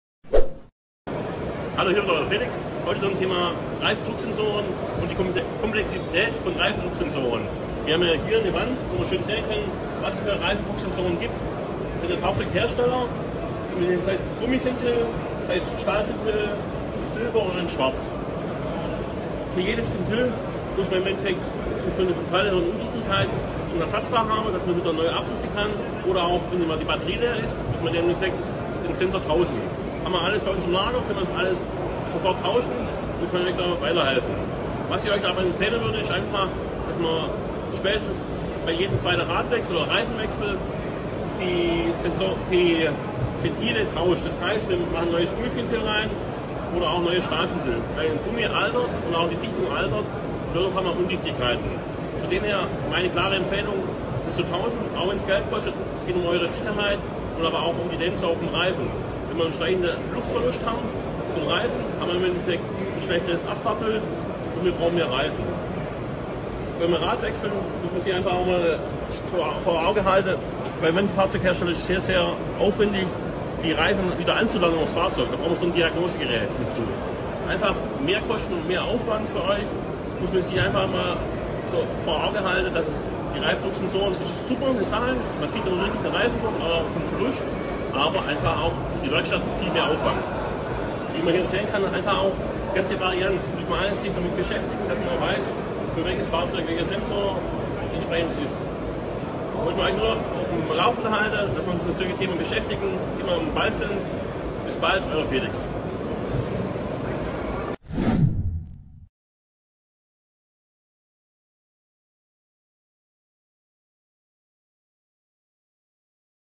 Wir erzählen euch einiges über RDKS Sensoren Reifendrucksensoren Sensor Reifendruck Luftdruck Reifen
Wir erzählen heute euch einiges über Reifendrucksensoren, auch RDKS System genannt 🙂 viel Spass dabei

Fahrzeugtechnik Bossert
Ziegelburgstr.9
72160 Horb
Tel. 07451/3075